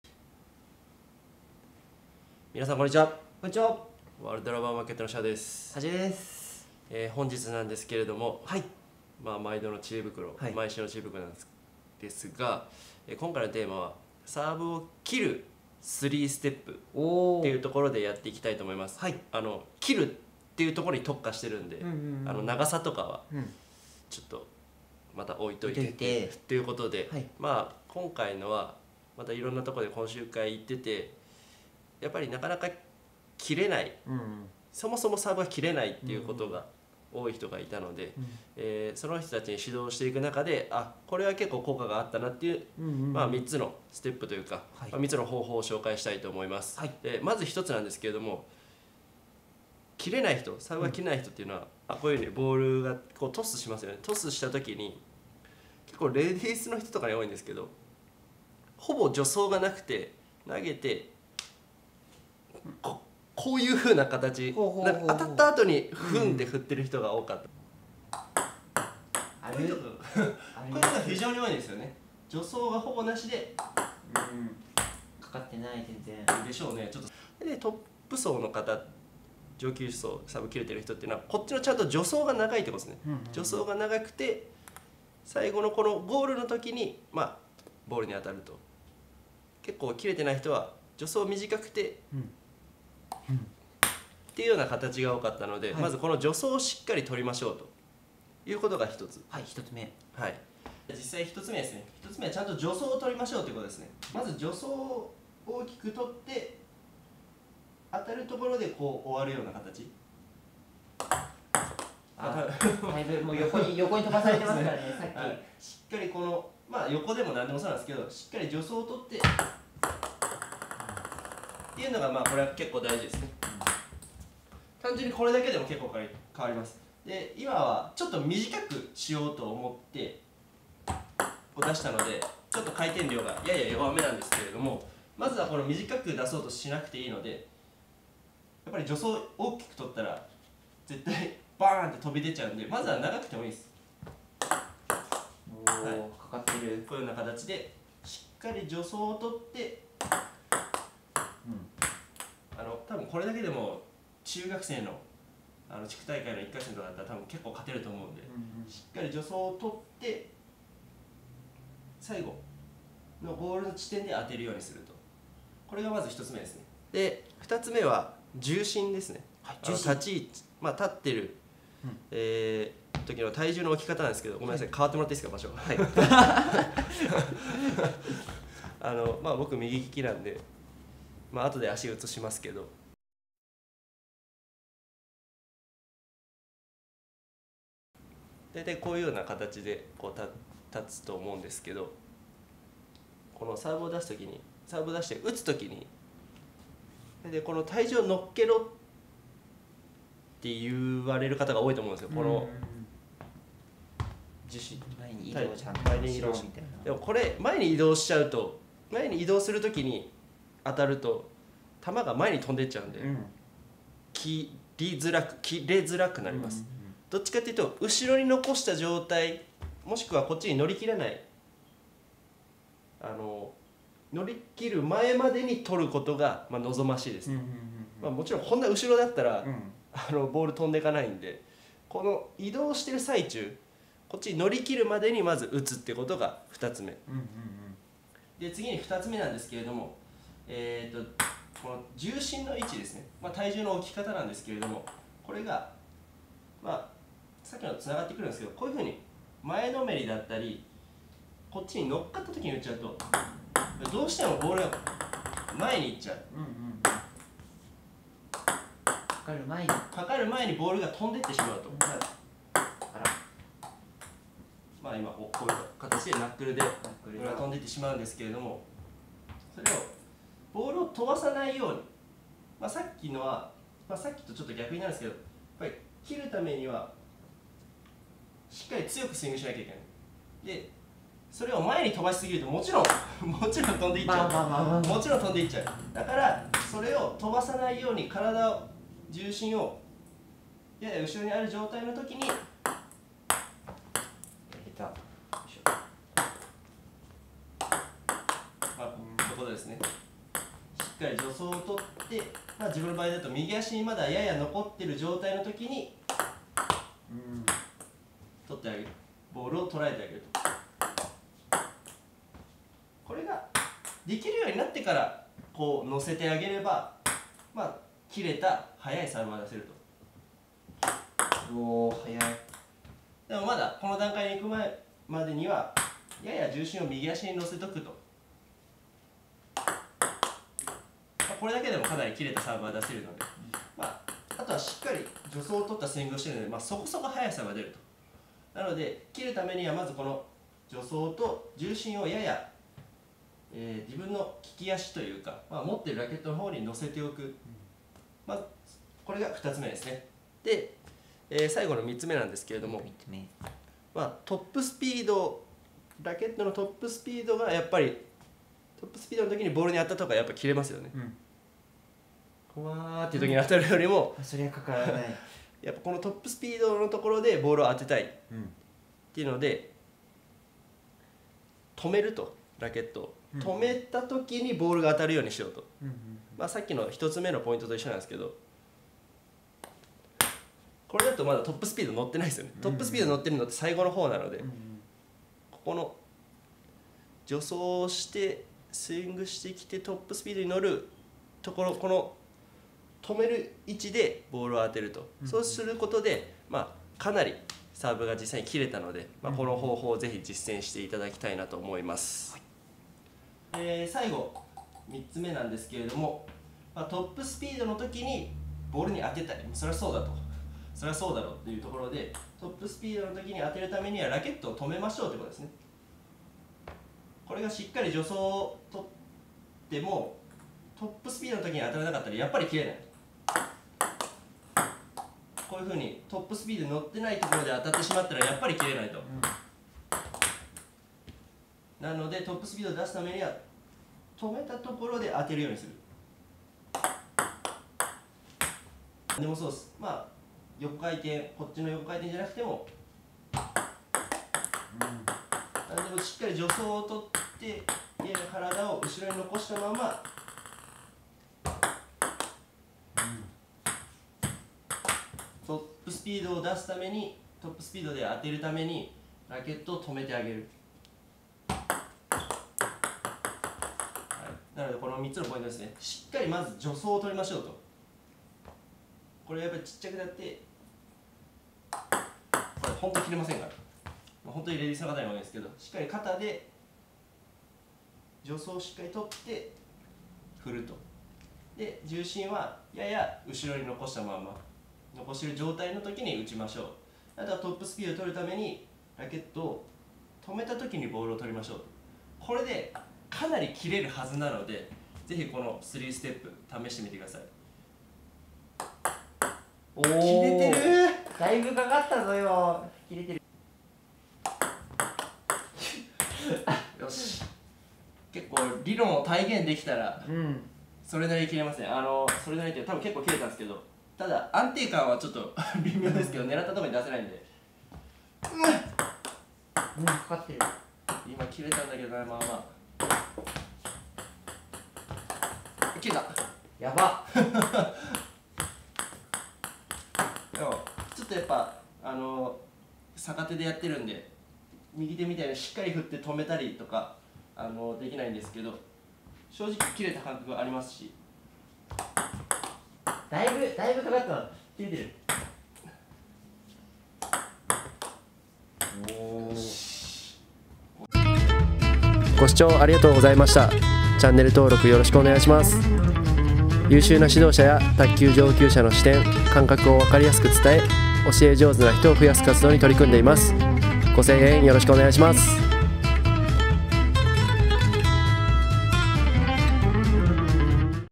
切れたサーブ3ステップ習得法！【卓球知恵袋】
みなさん、こんにちは！WRMのXiaです。本日は最近の講習会で効果のあった『切れた』サーブ3ステップ習得法について紹介します。みなさん、色んなサーブを覚えたいと思いますがまずは「切れた」サーブを絶対覚えたいですよね？

この方法はペン・シェーク関係ない、戦型も関係ない、巻き込みや順横、バックサーブなど色んなサーブの形にも応用できる。といった「切る」サーブの考え方を3ステップにして誰にでも覚えやすいようにした動画になります。
〇ステップ１
しっかり助走(バックスイング)をつけよう
〇ステップ２
重心を利き手側に残そう
〇ステップ３
トップスピードで打つためにラケットを止めたところで打球しよう
ステップ１、２、３の方法を簡潔に紹介したどんな戦型にも応用しやすいサーブ講習動画になっていると思いますので是非動画をご覧ください。この動画が何かの参考になれば幸いです。